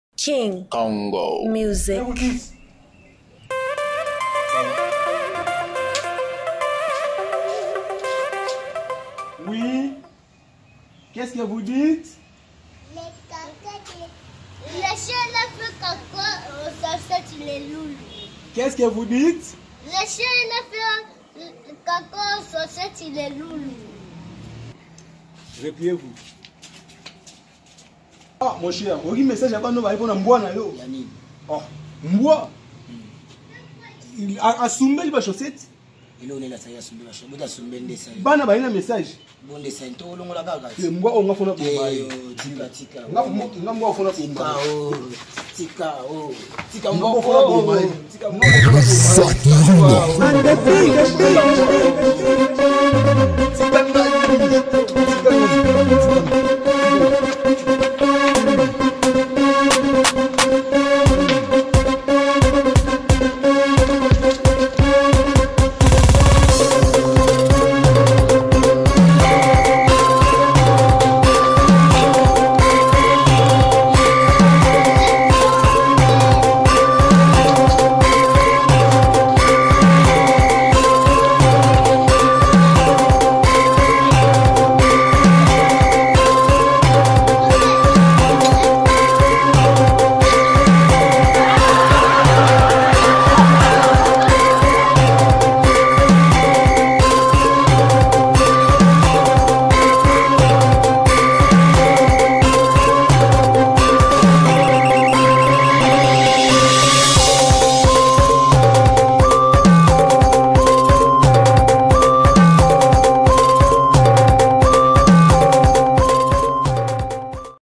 Chien Larato
Le Chien Lerato qui pèse des tonnes!💪🏾. Asali makambo